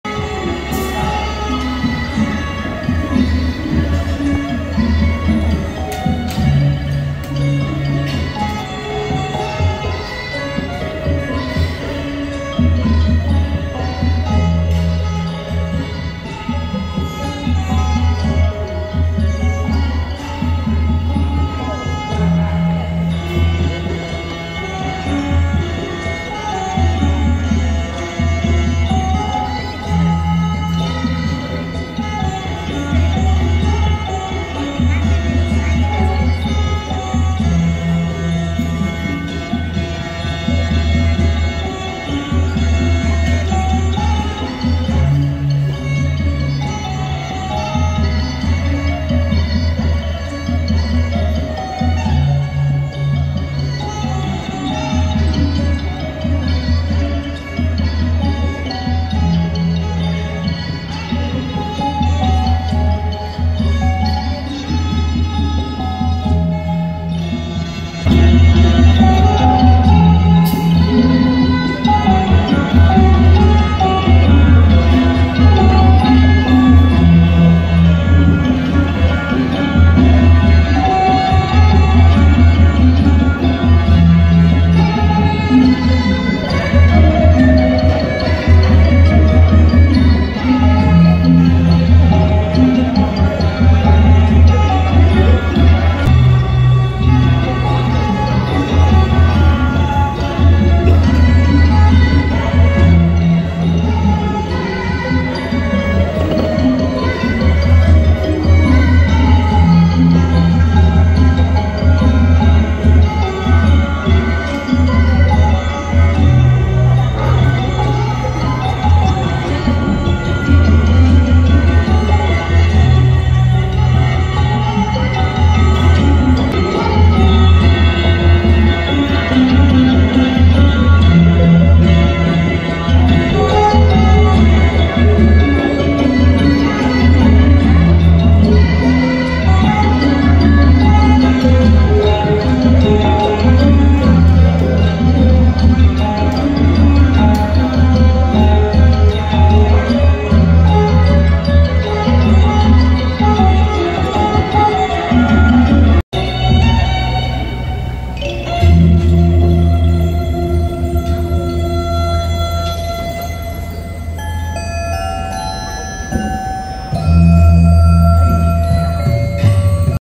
Thai Dance Show
Amazing Thai dance show by the river at Riverside Terrace, Anantara Riverside Bangkok Resort